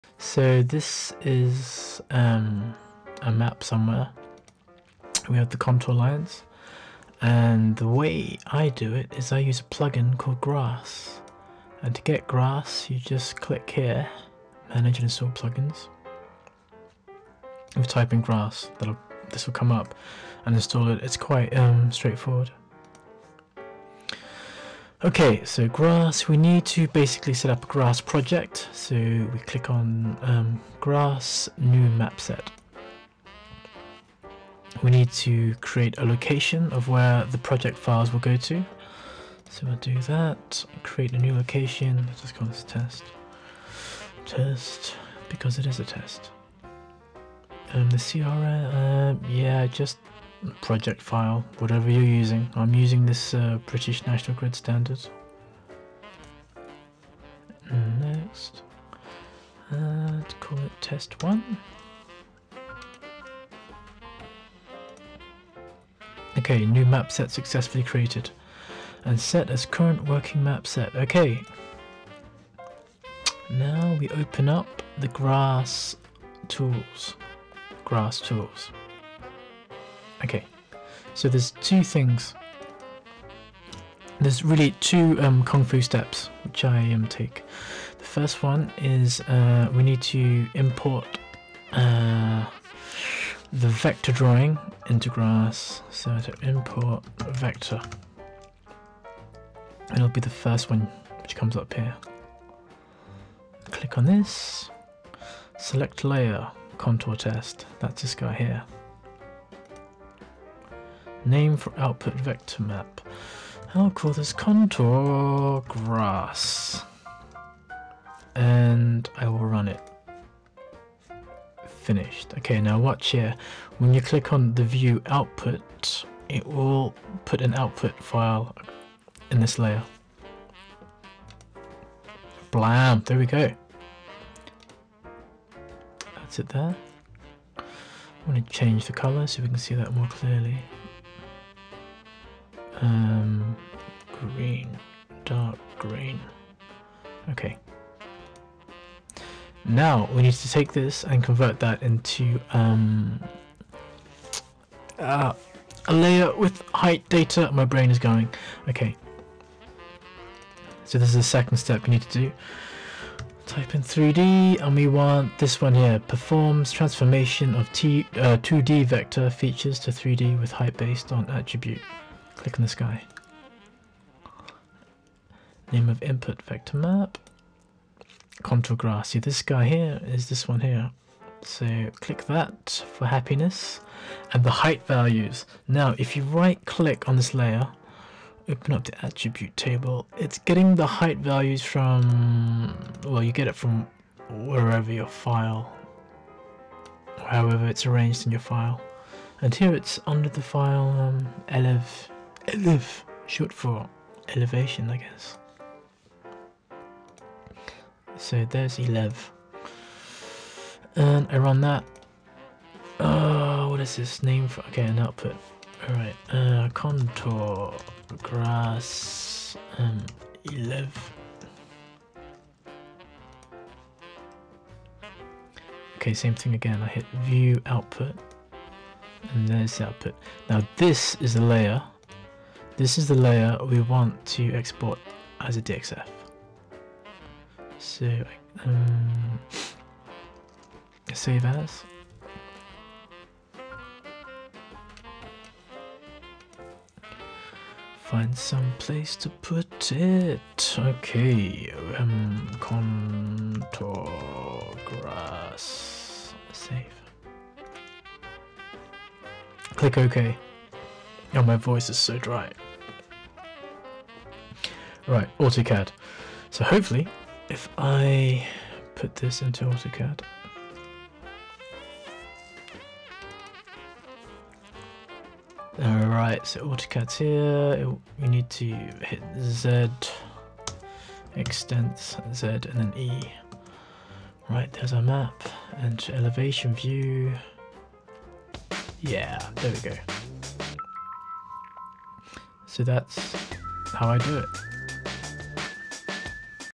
QGIS: exporting contours to DXF with height values
This might be useful for anyone in who needs to go from analysis (GIS) to construction and CAD (landscape architecture, planning etc).